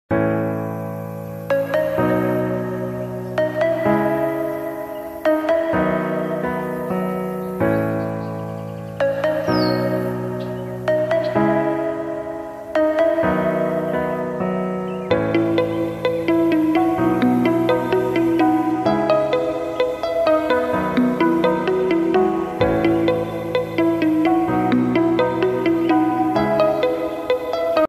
HIGH QUALITY SCREWS and FSATENERS in SIGMA STEEL FACTORY
How screw are made in factory? Our company SIGMA speacialized in stainless steel coil with long history. And our products gain porpularity consumers at home and abroad. stainless steel l#steel screw#china exports Trade import Factory# fasteners screws.